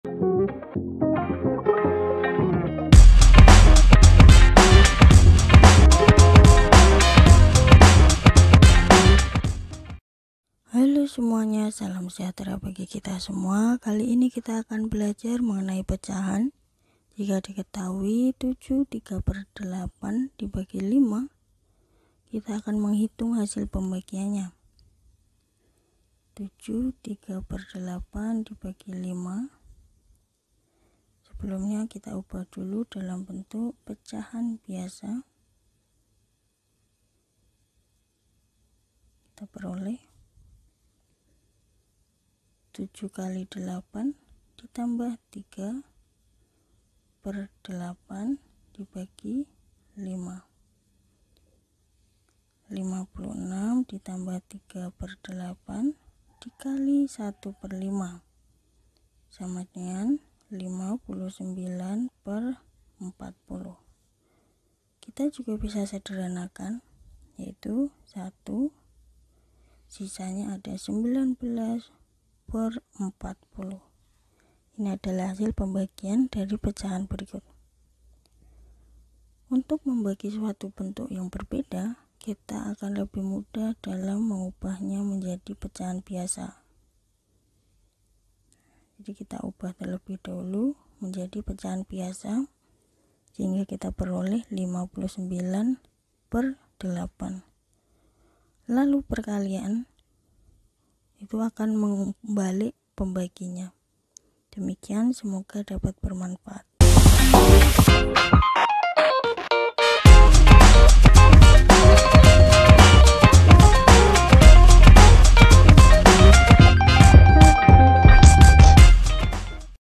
\begin{array}{l} \text { Selesaikan pembag... | Pintar Matemati | Kelas 6 | 5 | Matematika
✅ Dalam video ini kita akan membahas: \begin{array}{l}
\text { Selesaikan pembagian pecahan berikut ini. }\\
\text { 18. } \quad 7 \frac{3}{8}: 5=\ldots
\end{array}
Tema:
Kelas: 6
Mata Pelajaran: Matematika
Bab: 5
Buku: Pintar Matematika 6B SD Kelas VI

👉 Buku yang telah tersedia:
{PLAYLIST_LINK}
Di mana kamu dapat menemukan kami?